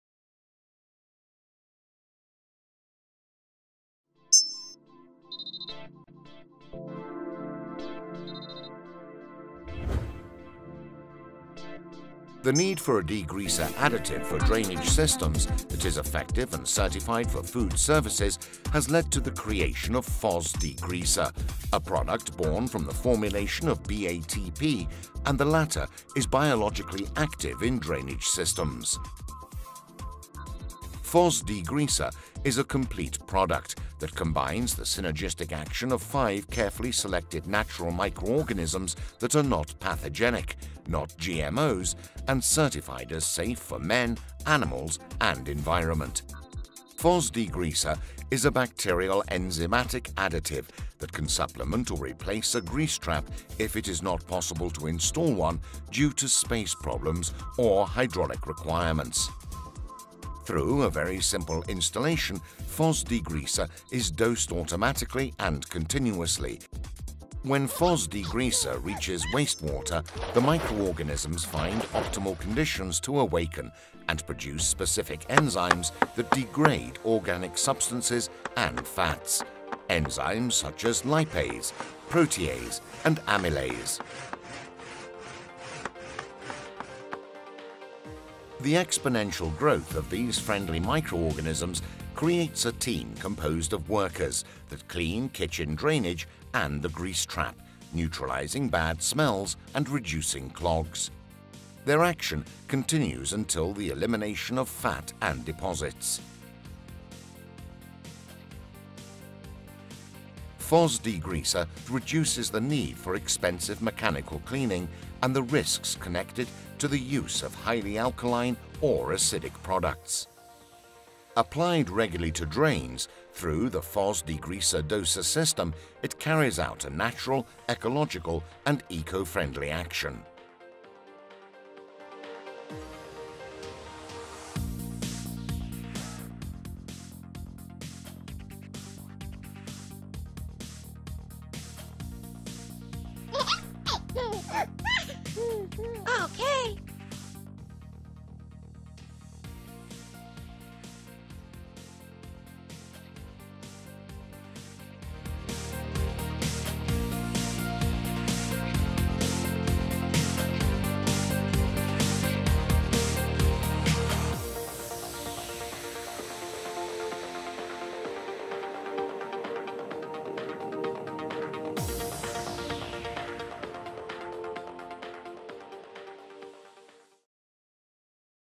FosDegreaser ENG
Biological activator born from BATP® , international patent that thanks to its particular application eliminates clogs and encrustations in all kind of sewage systems.